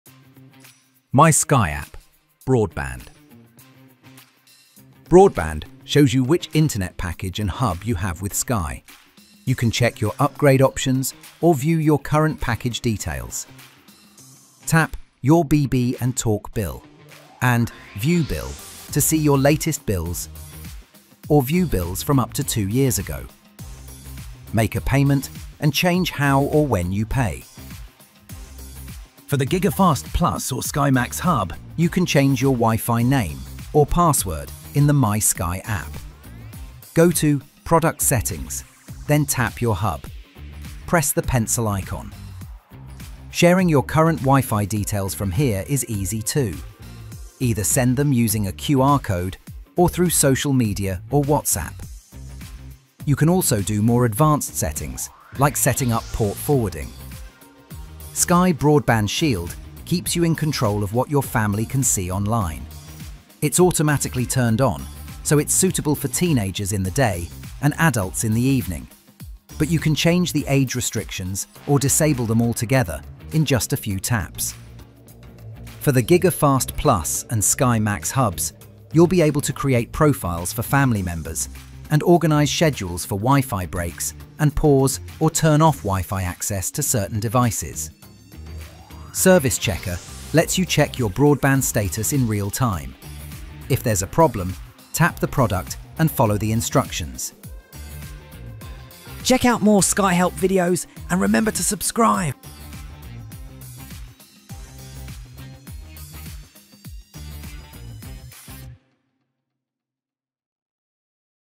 Use the My Sky app to manage your Sky Broadband - Sky Help
Manage your internet package, WiFi settings, security features, and more—all from your phone.

🌐 Broadband Overview

See which Sky Broadband package and Hub you have
View your current plan or explore upgrade options

💳 Bills & Payments

Access up to 2 years of billing history
View upcoming bill estimates
Make payments or update your payment method and date

📶 WiFi Settings

Change your WiFi name and password
Share WiFi details via QR code, social media, or WhatsApp
Access advanced settings like Port Forwarding

🛡️ Security & Parental Controls

Manage Sky Broadband Shield to block unwanted content
Set age restrictions or disable the Shield
Control your McAfee subscription
Create profiles for family members
Schedule WiFi breaks or block access to specific devices

🛠️ Real-Time Service Checker
Check your Broadband status instantly. If there’s an issue, tap the product and follow the instructions.

📱 Download the MySky app from the App Store or Google Play